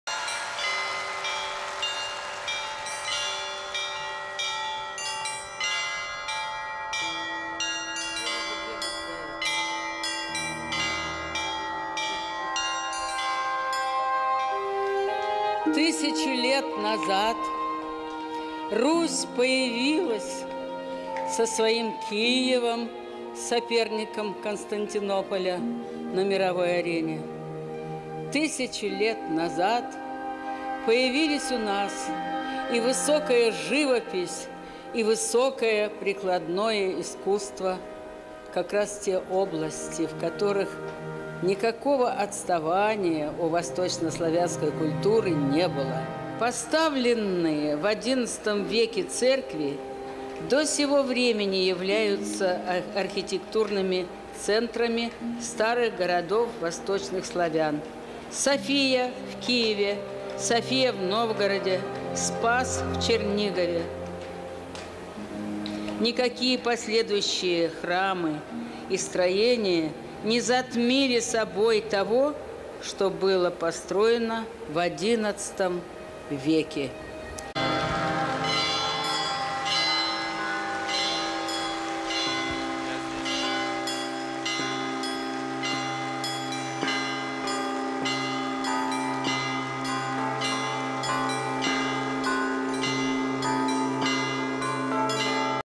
From Rus' to Russia - 05 - Baptism of Rus' and the State of Rus'
Day of Russia 2015, a grand concert on the Red Square

Baptism of Rus' and the State of Rus'
Elina Bystritskaya
[fragment of article by D. Likhachov]